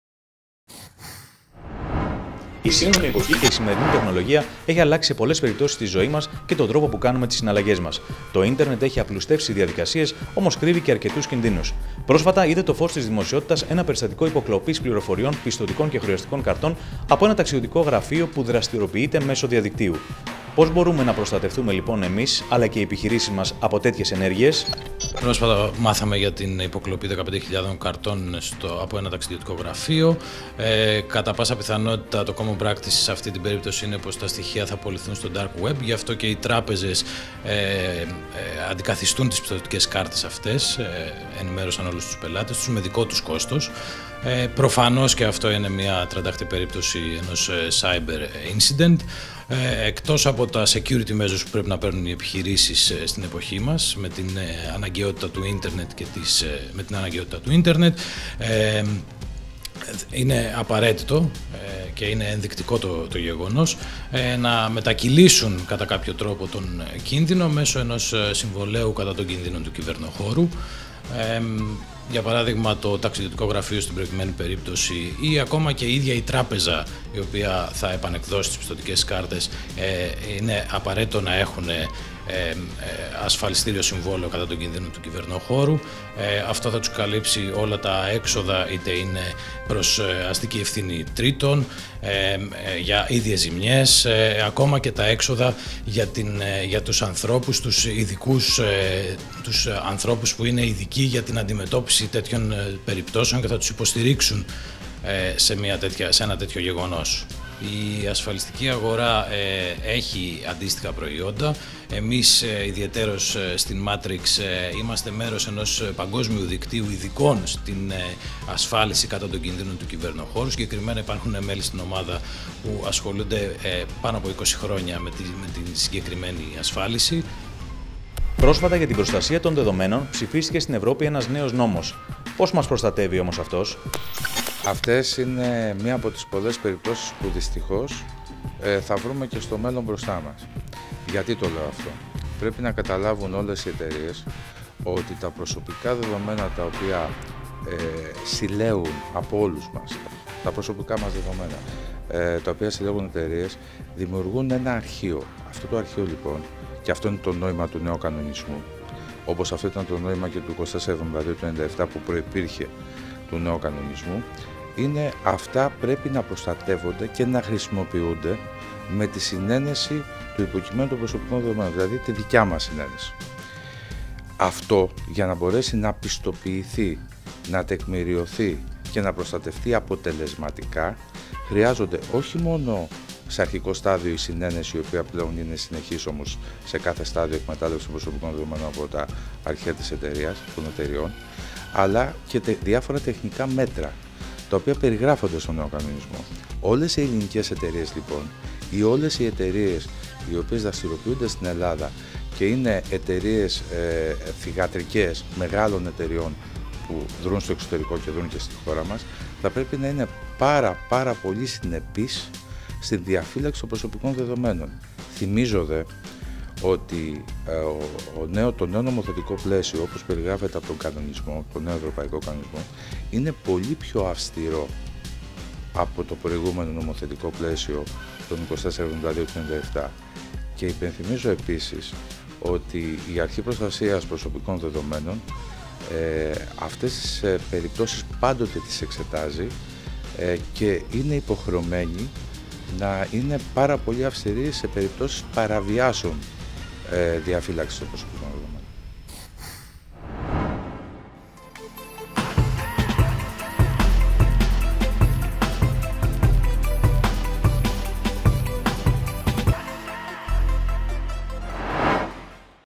Ασφάλιση των κινδύνων του κυβερνοχώρου
Υπάρχει ασφαλιστικά αποτελεσματικός τρόπος να θωρακιστεί μια επιχείρηση απέναντι στις κυβερνοεπιθέσεις και να ελαχιστοποιήσει τις επιπτώσεις αυτής;